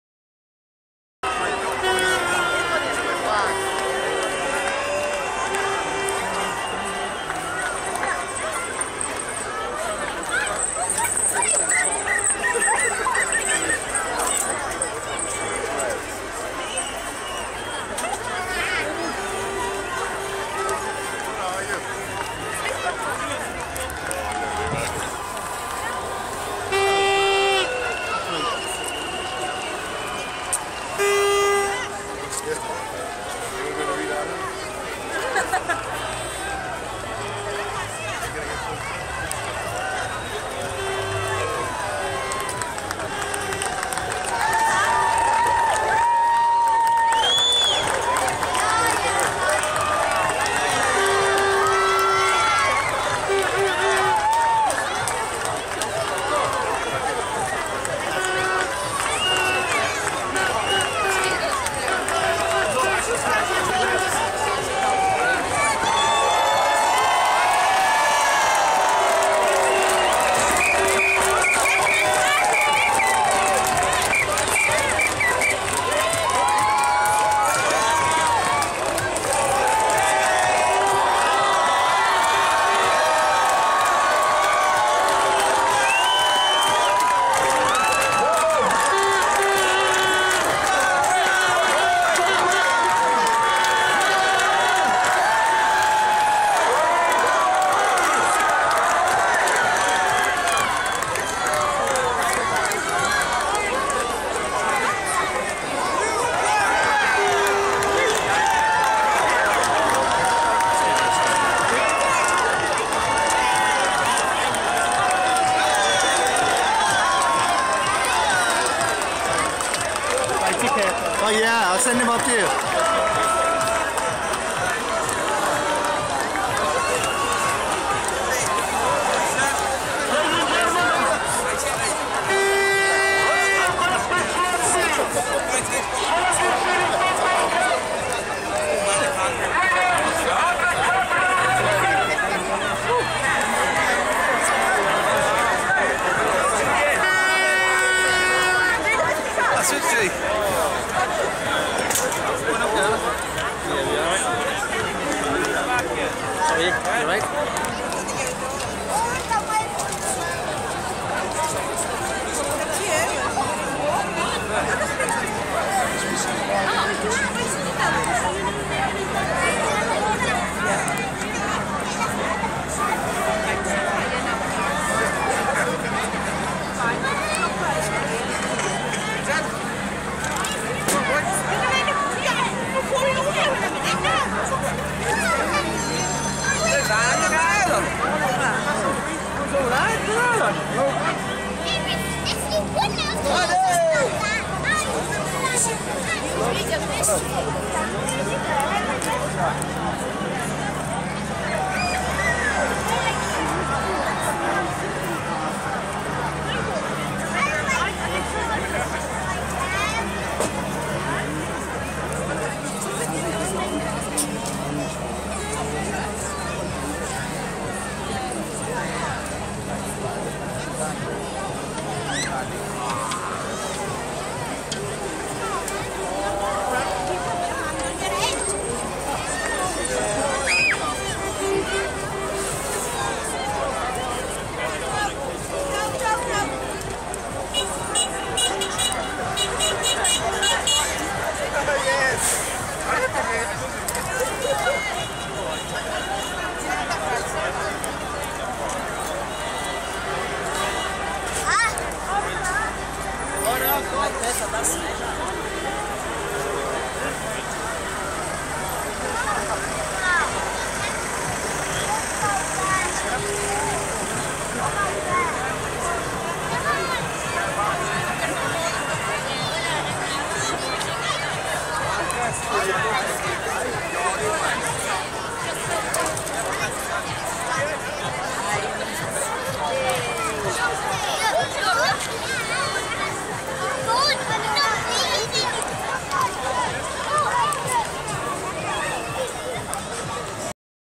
The Swans Celebration Video Clip
Swansea City Football Club's Celebrations and bus ride where it officially started at the Dragon Hotel on the Kingsway Swansea. There was a great atmosphere for the many who were able to turn out and every Swans fan around the world will I'm sure appreciated. God blessed us all with this victory which I am sure more is to come!